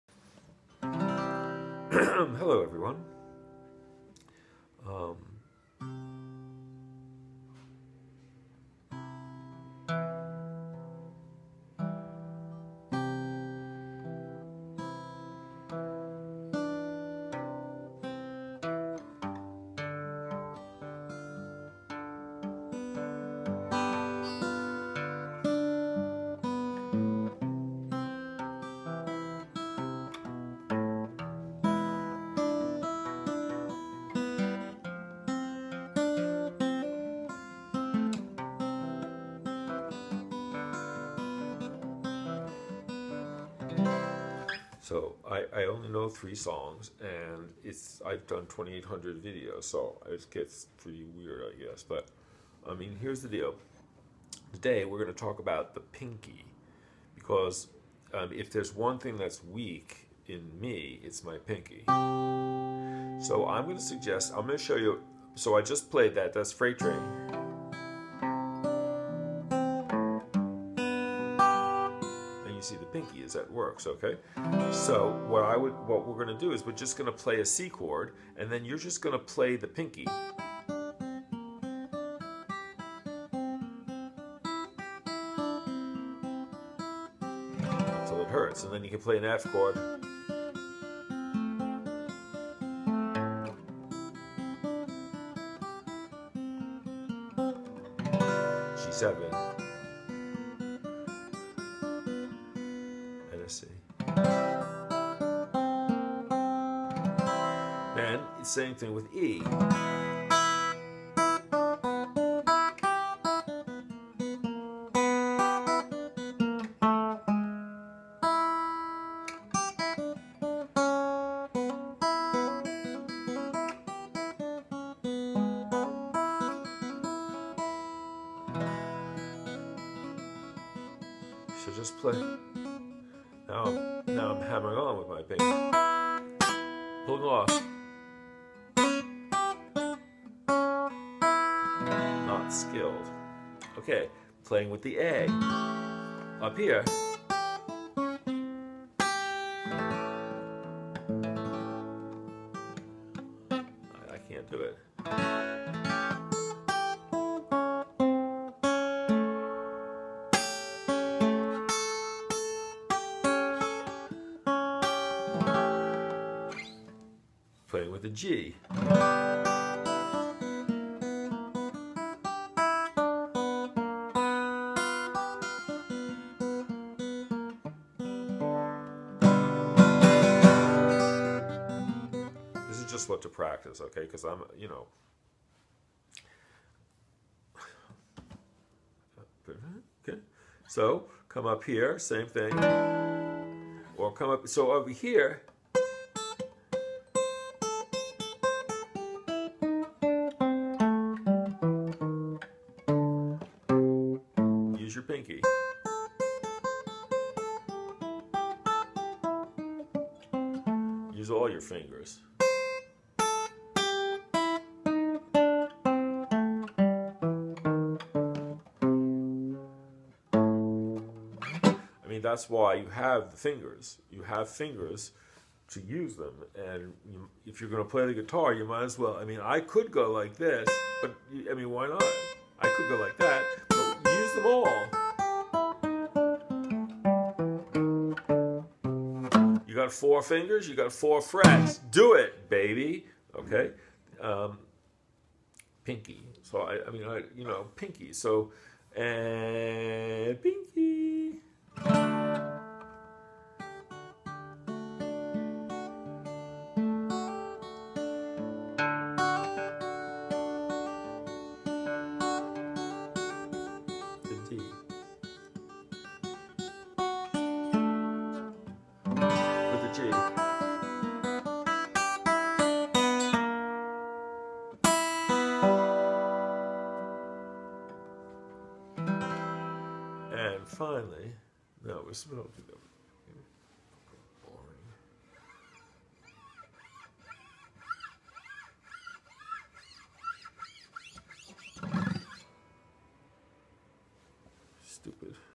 Using The Pinky Finger While Playing Guitar
Hello sweethearts! I do this for fun and to spread happiness and Love!    Guitar Lesson. Blues Guitar Lesson.

FINALLY, PRACTICE.